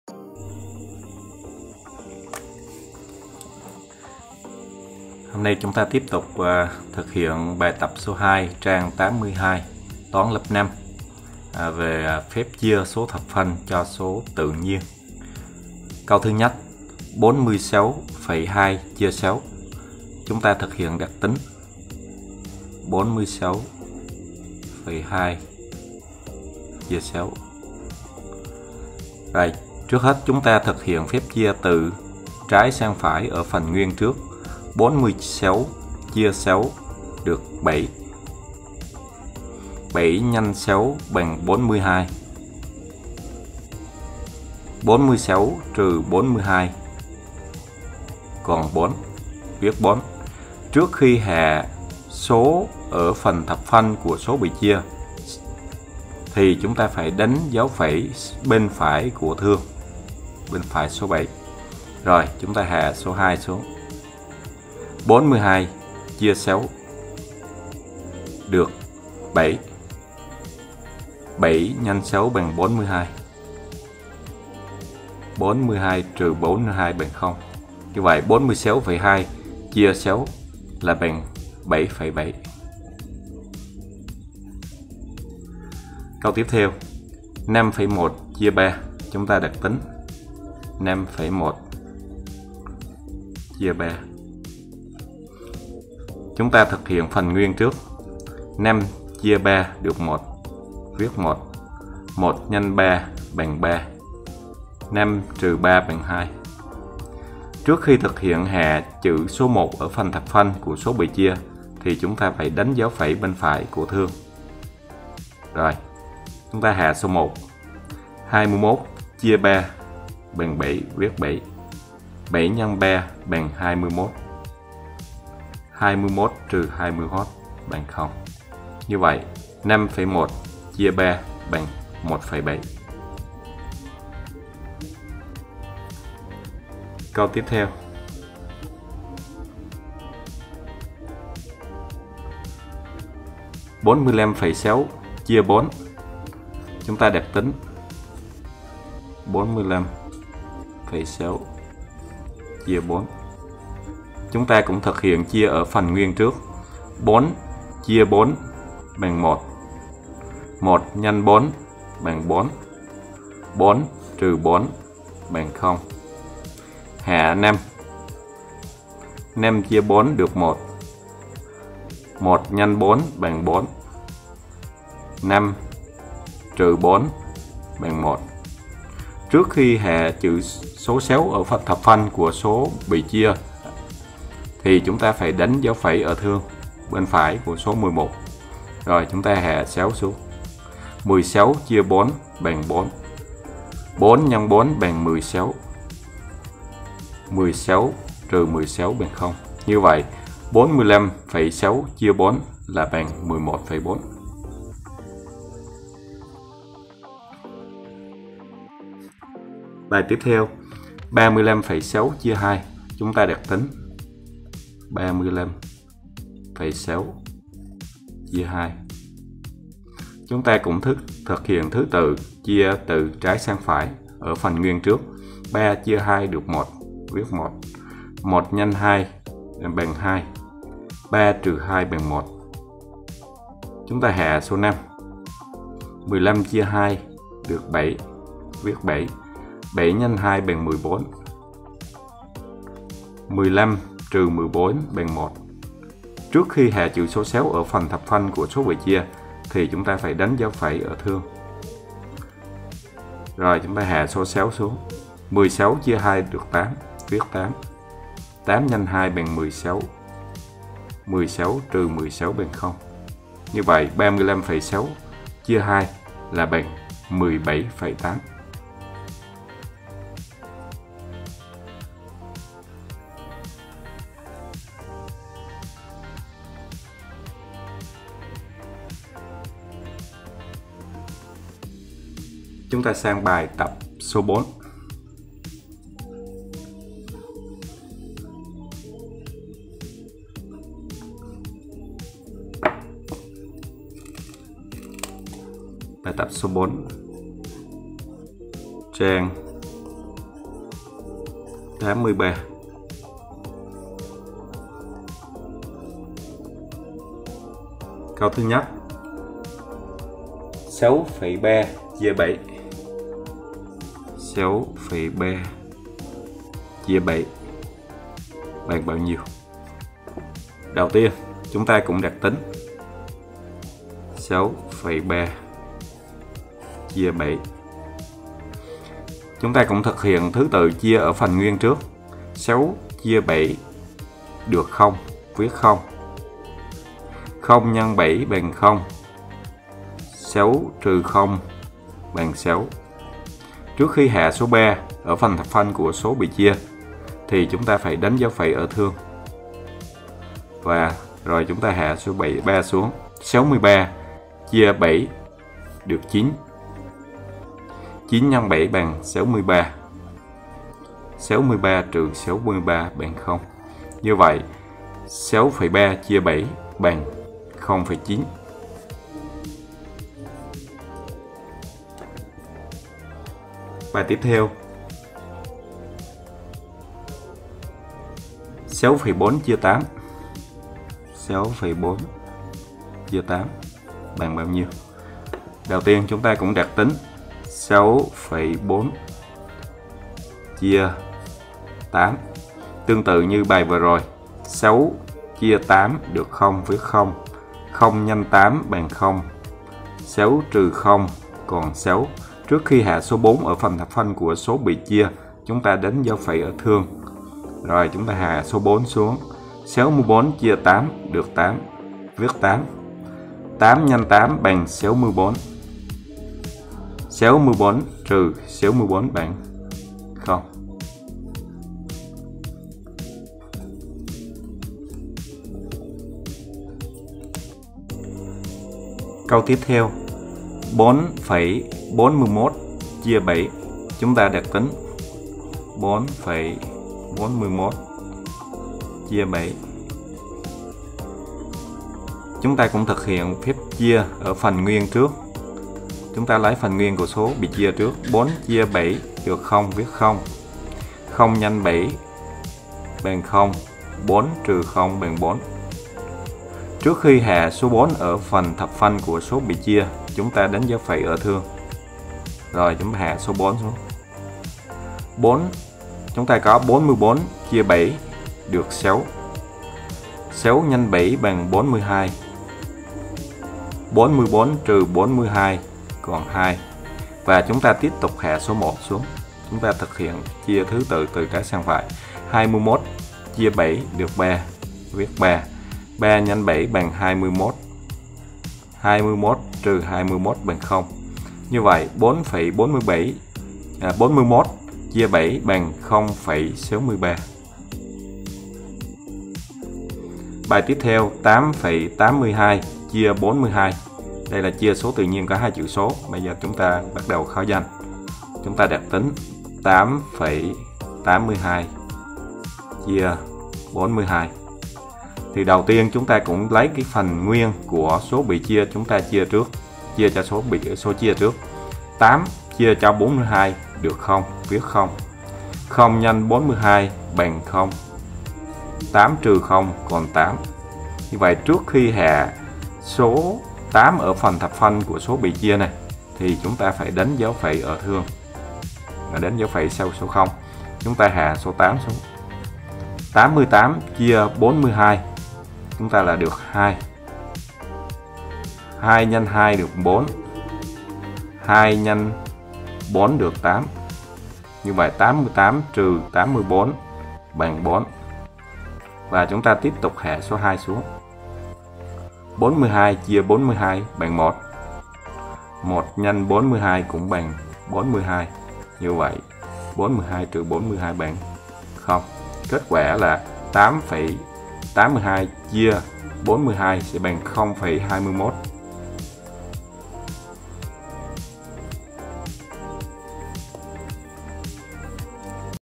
Phép chia số thập phân cho số tự nhiên (tt), bài 2, bài 4 trang 82 [Toán 5] |Trường học thân thiện
Hướng dẫn cách thực hiện phép chia số thập phân dạng đơn giản; bài tập 2, bài tập 4 trang 82, 83 sách giáo khoa Toán lớp 5 bộ sách Cánh diều